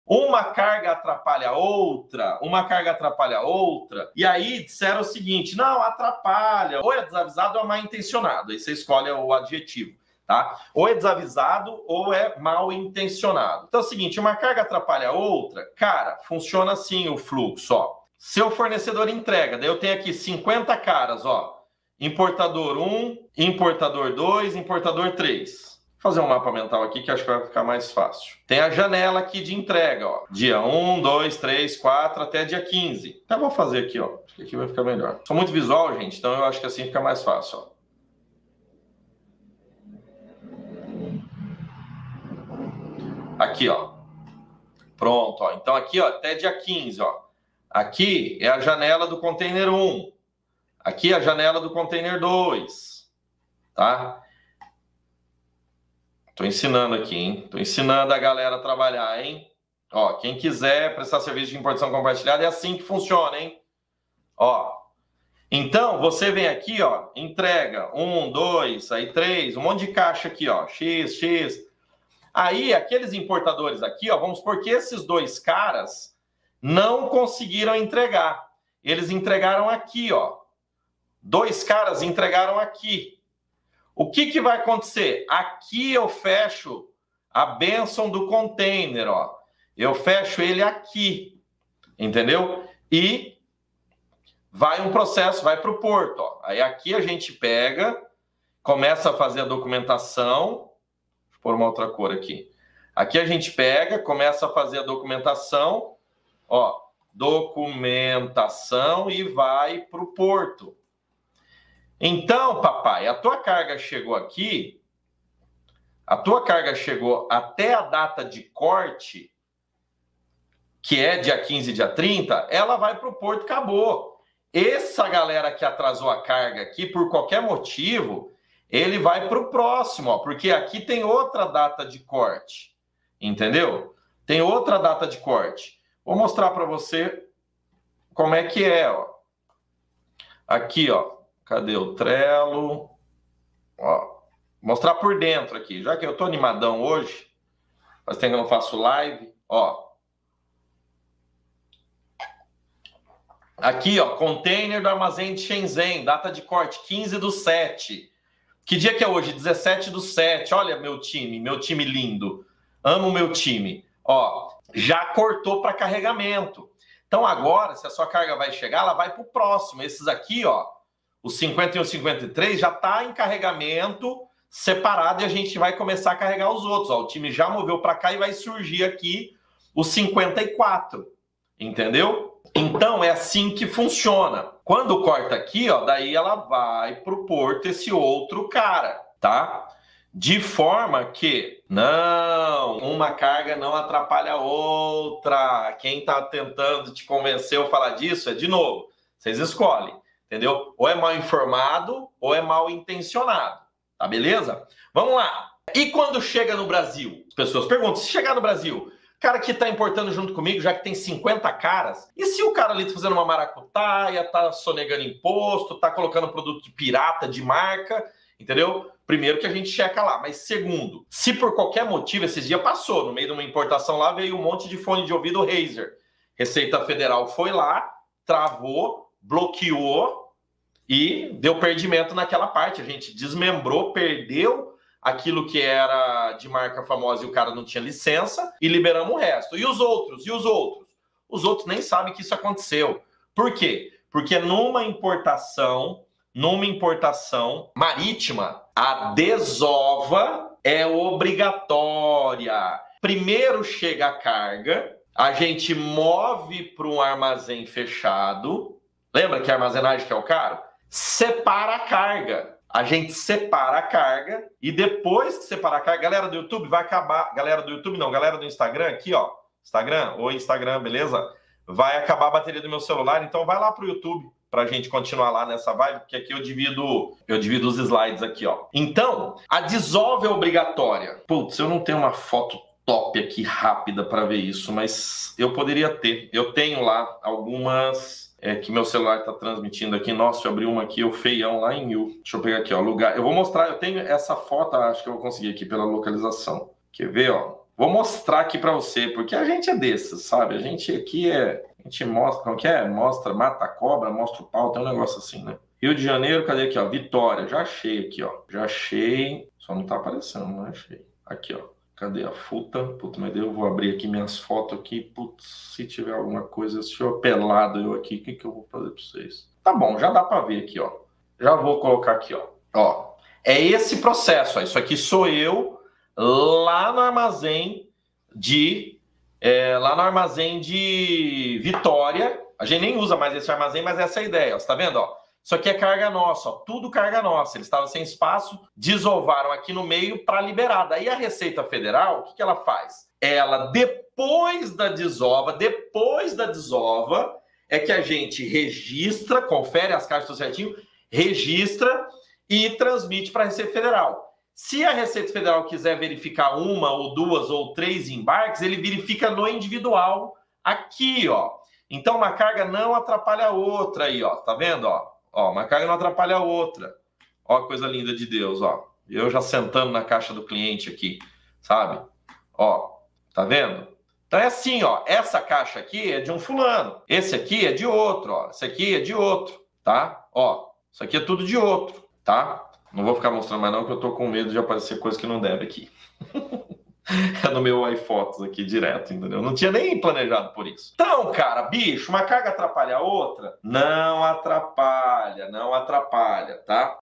UMA CARGA ATRAPALHA A OUTRA NA IMPORTAÇÃO COMPARTILHADA?
Nesse vídeo Rodrigo quebra o mito de que uma carga atrapalha a outra em uma importação compartilhada.

Isso não acontece pois o container tem data de corte para ser finalizado e carregado.

Então se uma carga de um importador não é entregue no nosso armazém, essa carga vai ser embarcada automaticamente no próximo container. E se sua carga está no depósito, ela será embarcada normalmente.

Em relação à liberação no Brasil, acontece a mesma coisa.

Pois as cargas são descarregadas e separadas antes de registrar a DI. Então a fiscalização e liberação na Receita acontece de forma individual.

É assim que reza o procedimento da Receita Federal e é assim que fazemos na China Gate.

Vamos com tudo e bora importar.

📦  IMPORTAÇÃO DIGITAL
Container Compartilhado com saídas quinzenais.